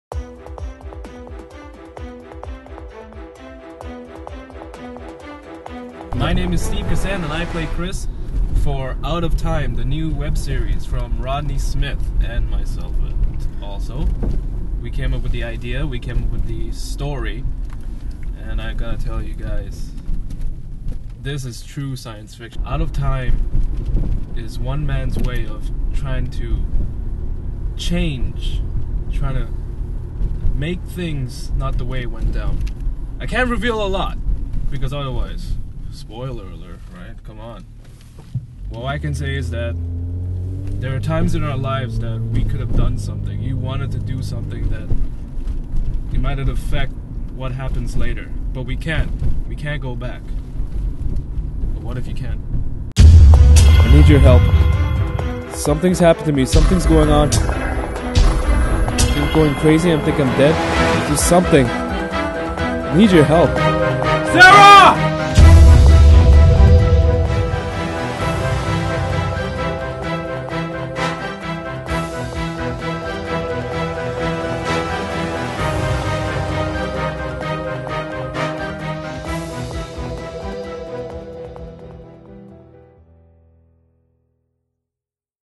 Production Blog 3: Out of Time
Pre-Production continues! Steve Kasan talks about OUT OF TIME! Includes a special promo for the upcoming series! With Dulce Felix, Steve Kasan, Shailene Garnett and Rodney V. Smith.

Shot and edited by Rodney V. Smith
Out of Time is created by Rodney V. Smith and Steve Kasan.
Produced by Flyin Monkey Films and GD Production.